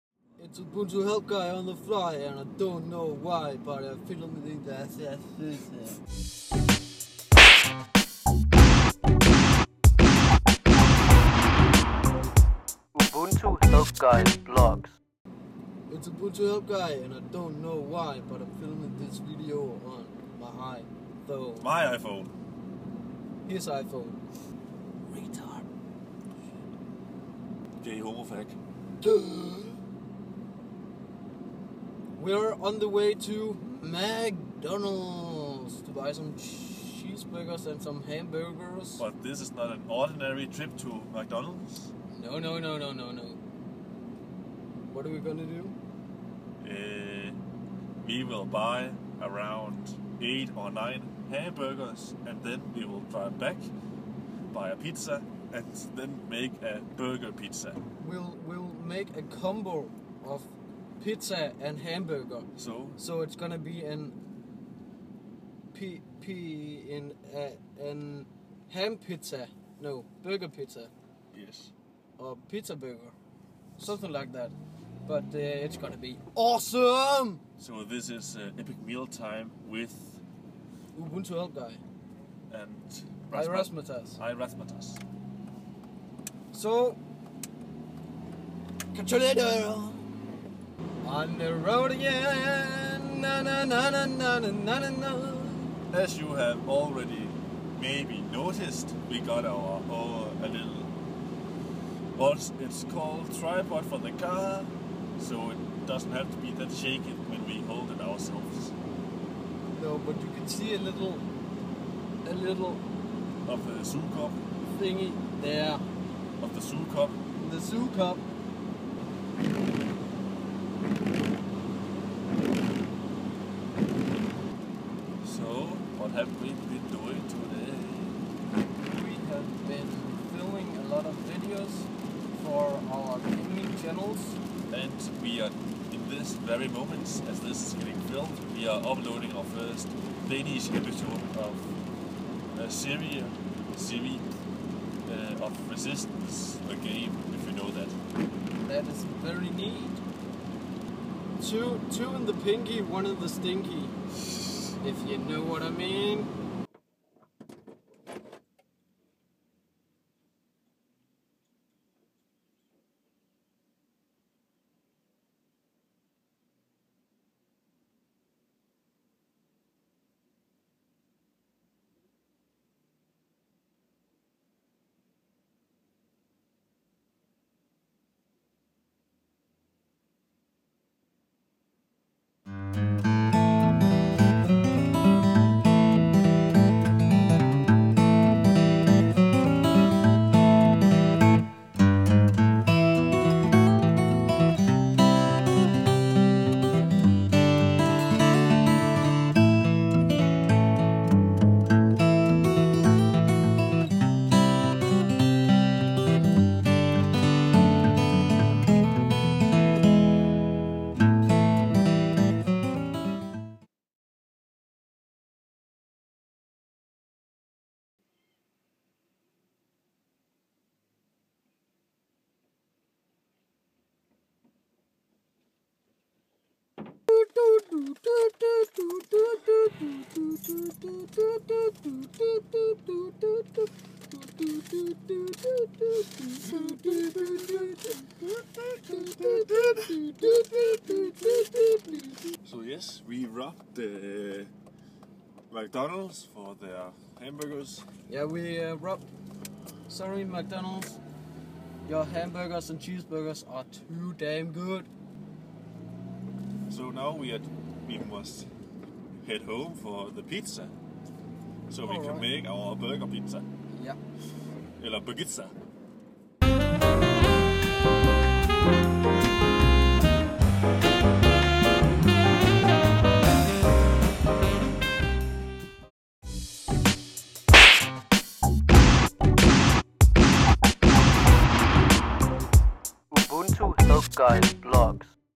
HAMBURGIZZA ROBBERY!
This is the vlog, where we rob McD and create a ''Burgizza'' - Uhhhh, exciting! Haha, hope you'll enjoy :)